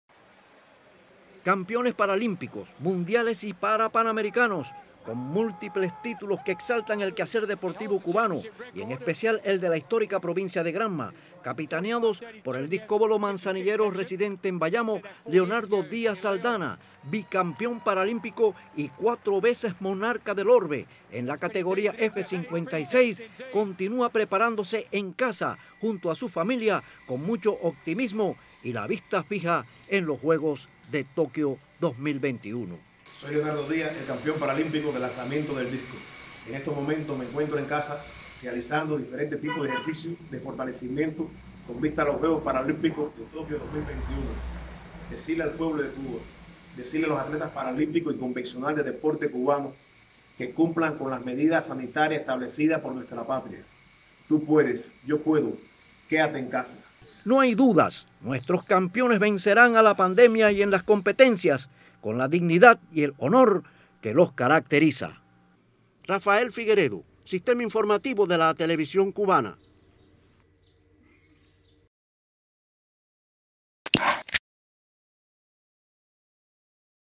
Desde casa se prepara el campeón paralímpico Leonardo Díaz
Nuestros campeones no pierden un ápice en sus aspiraciones por seguir dándole a la patria y a la provincia los éxitos en cada una de sus disciplinas en los deportes para adaptados de la ACLIFIM, luchando por la vida en momentos difíciles debido a la Pandemia de la Covid-19.